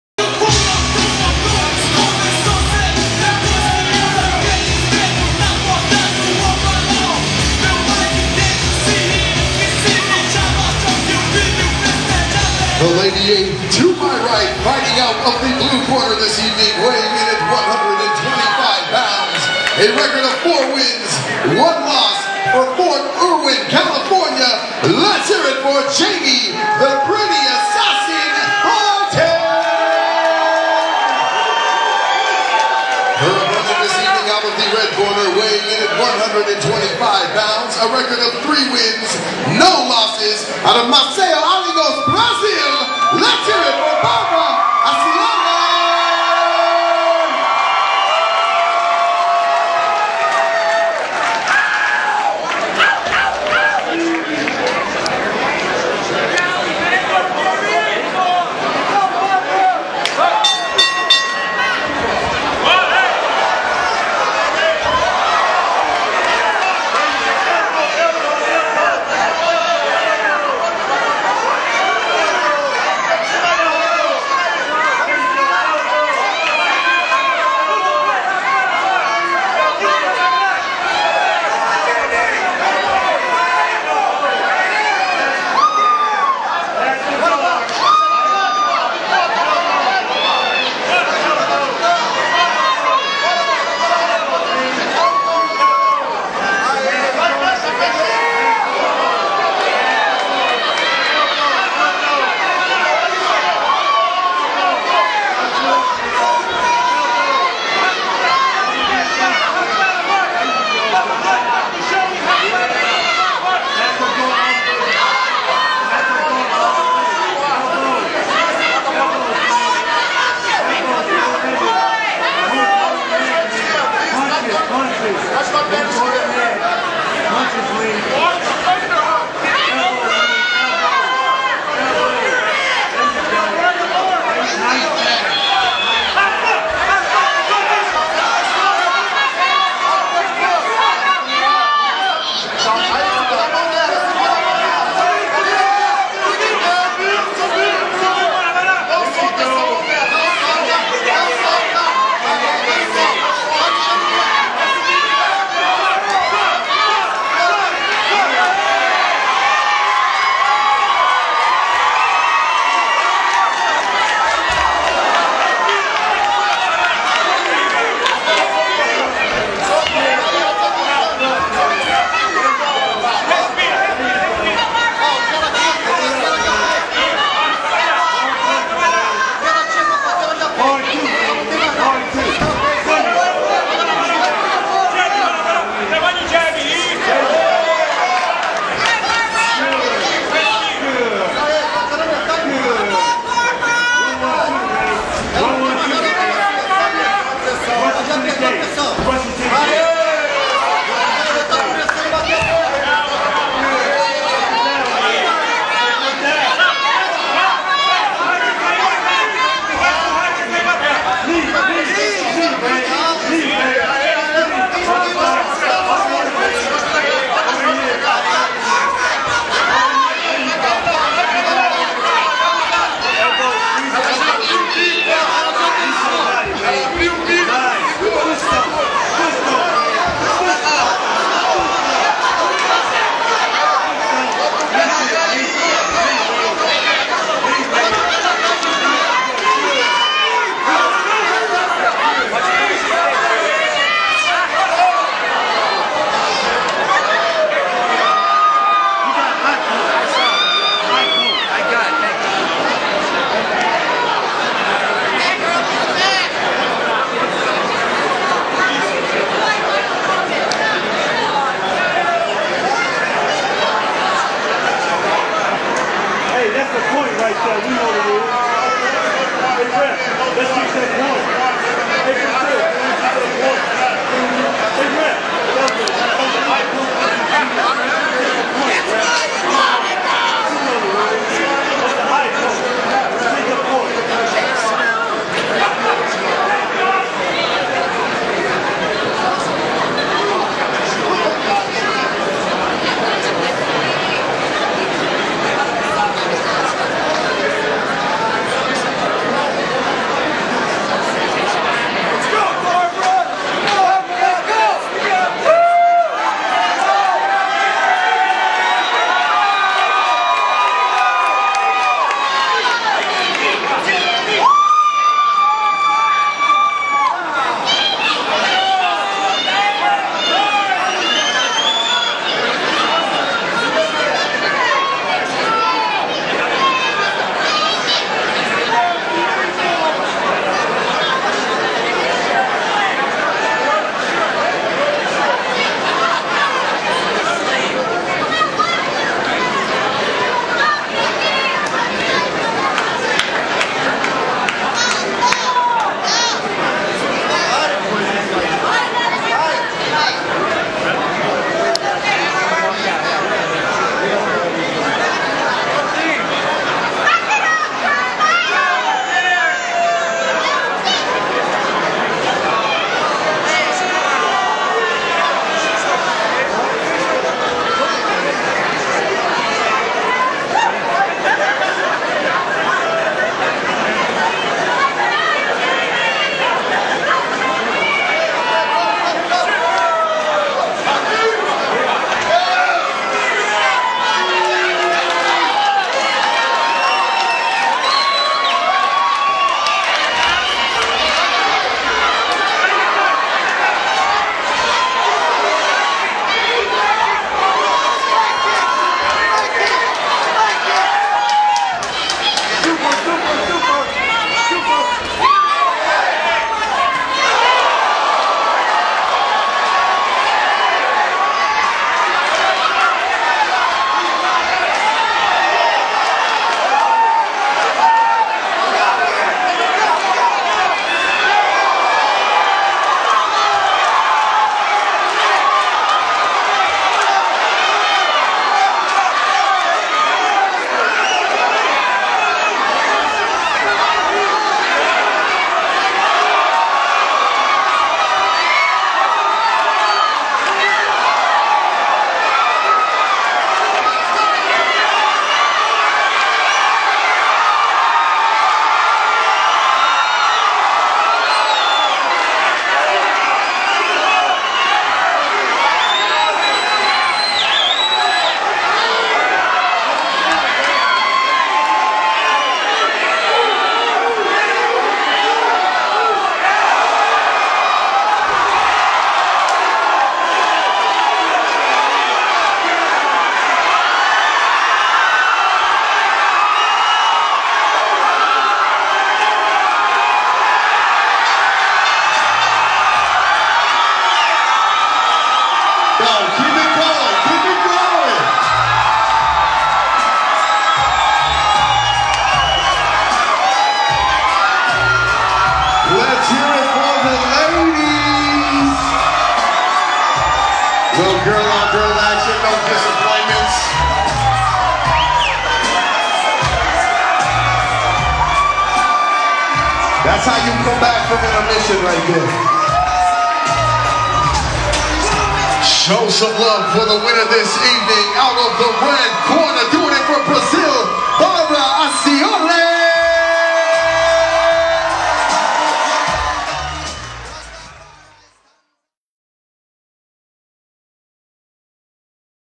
GKO 9 - Bárbara Acioly vs Jamie Thorton
Confira o vídeo da luta da alagoana Bárbara Acioly contra Jamie Thorton, no GKO9 em Sacramento (EUA)